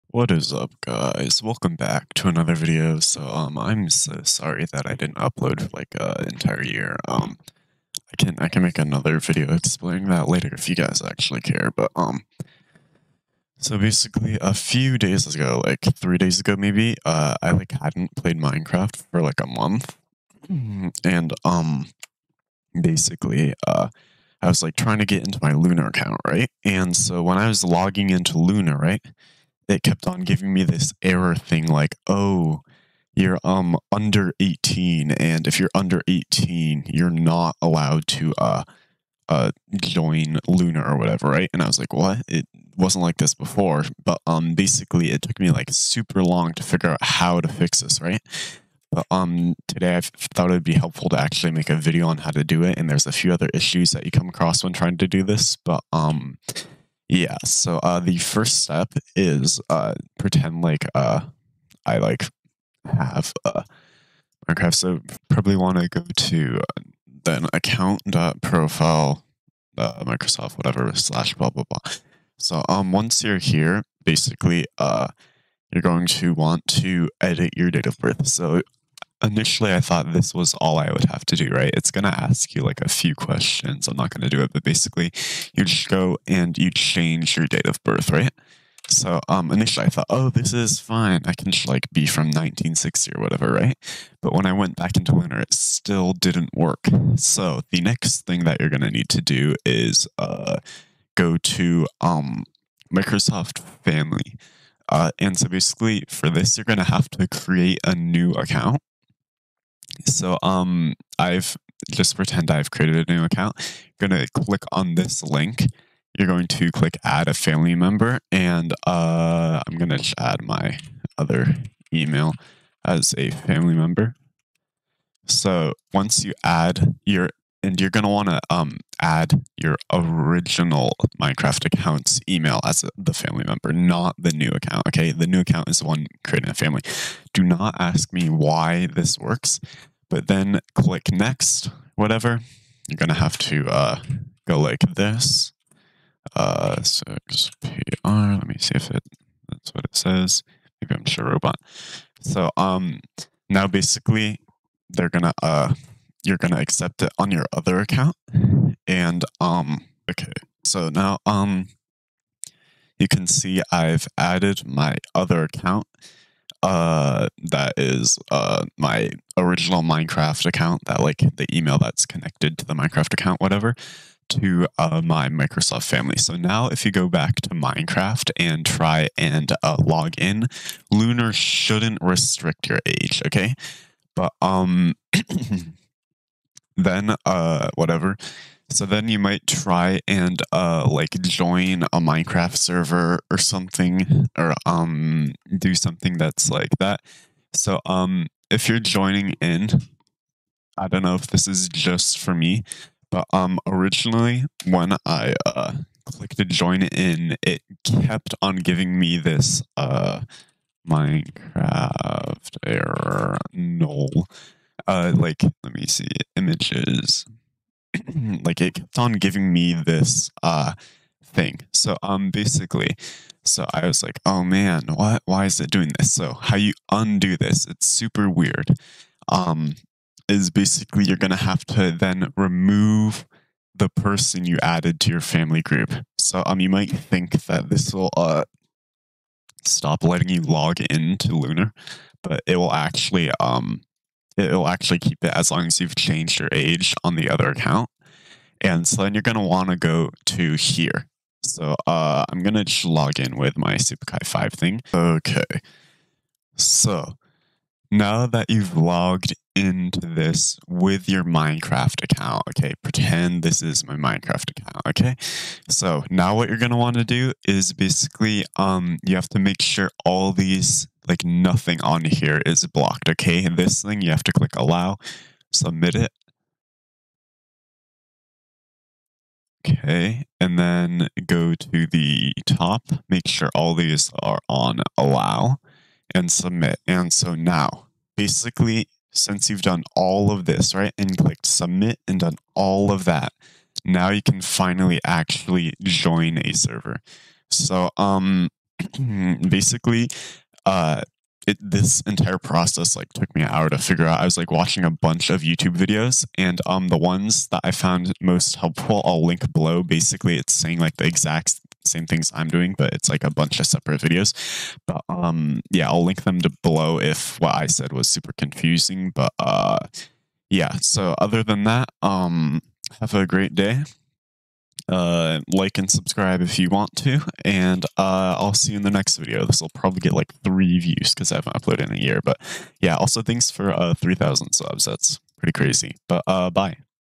How to Bypass Lunar Client Age Restriction and fix the null error
hi guys, sorry for not uploading for a year, tabnks for 3k subscirberts. I hope yall find this video helpful bc this issue took me a while to solve.